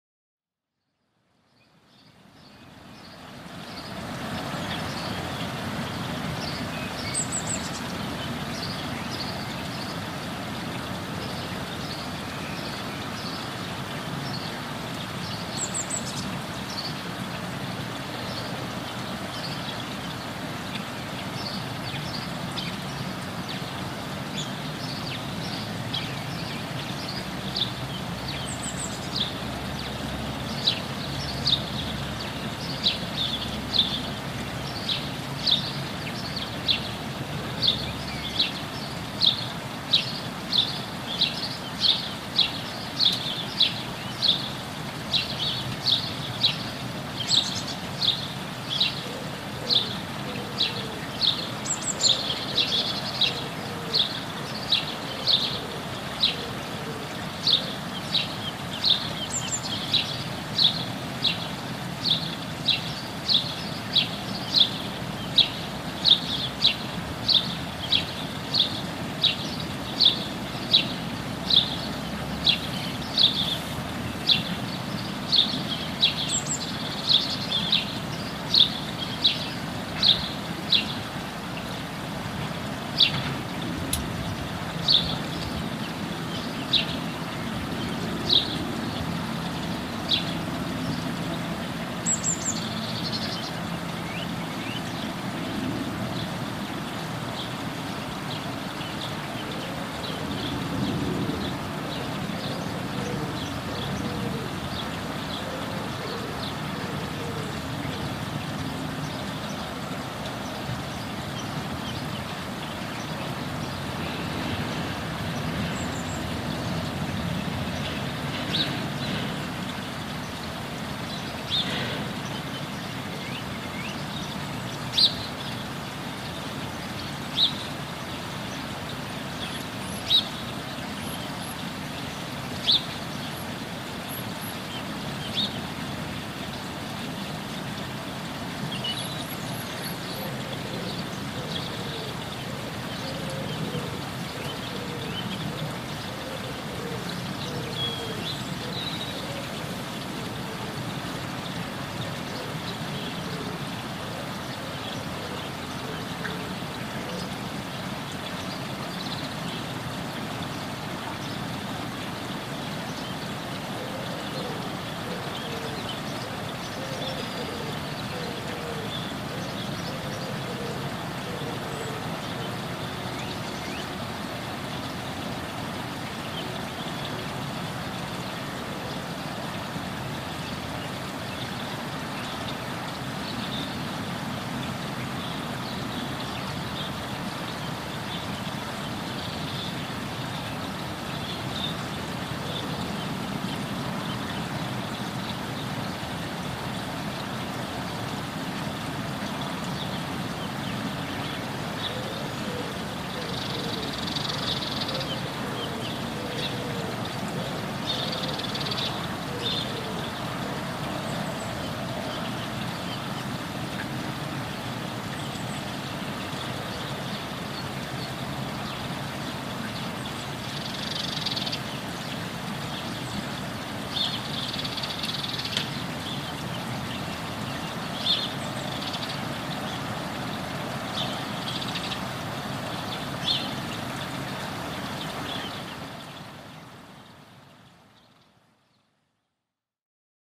dawn chorus in Walthamstow
Early morning sounds on wednesday 27/4/22 in London E17.